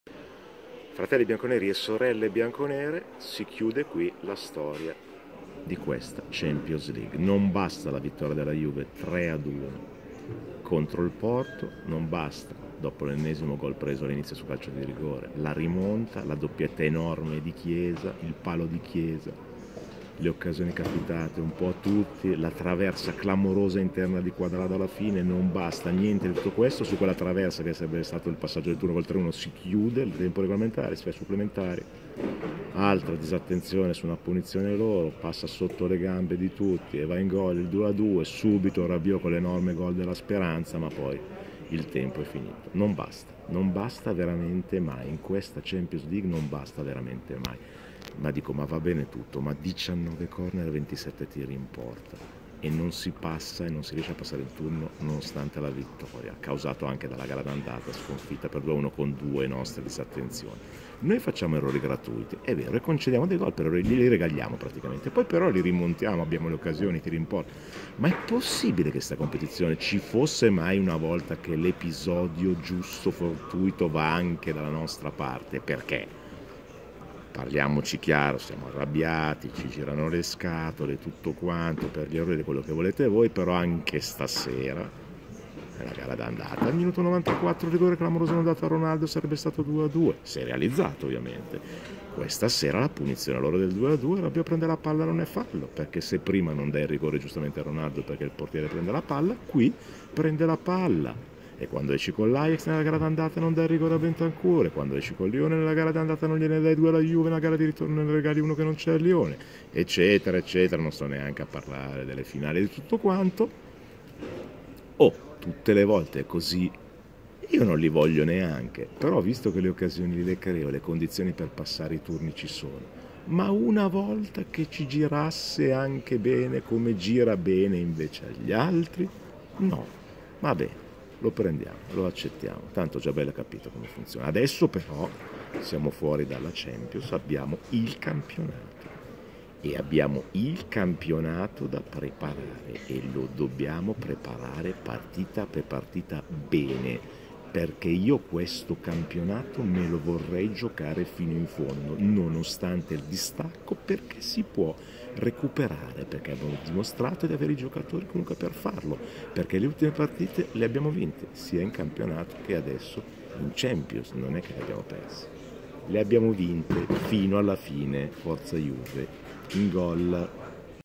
Juve Porto 3-2 ma non basta
Non bastano 27 tiri , non bastano 19 corner , non basta la doppietta di Chiesa e il gol di Rabiot. In Champions non basta mai

JUVENTUS (4-4-2): Szczesny; Cuadrado, Bonucci (30' st De Ligt), Demiral, Alex Sandro; Ramsey (30' st McKennie), Arthur (12' pts Kulusevski), Rabiot, Chiesa (12' pts Bernardeschi); Morata, Ronaldo. A disposizione: Pinsoglio, Buffon, Chiellini, Di Pardo, Dragusin, Frabotta, Fagioli. Allenatore: Pirlo

PORTO (4-4-2): Marchesin; Manafà, Mbemba, Pepe, Zaidu (26' st Luis Diaz); Corona (18' sts Diogo Leite), Sergio Oliveira (18' sts Loum), Uribe (45' st Grujic), Otavio (16' st Sarr); Marega (1' sts Toni Martinez), Taremi. A disposizione: Diogo Costa, Anderson, Evanilson, Nanu, Fabio Vieira, F. Conceição. Allenatore: S. Conceição

ARBITRO: Kuipers (Ola)

MARCATORI: 19' pt rig. Sergio Oliveira (P), 4' st, 18' st Chiesa (J), 15' sts Sergio Oliveira (P), 17' sts Rabiot (J)

NOTE: Espulso Taremi (P) al 9' st per doppia ammonizione. Ammoniti: Chiesa, Cuadrado, Bernardeschi (J), Sergio Oliveira (P). Recupero: 4' pt, 5' st, 2' pts, 3' sts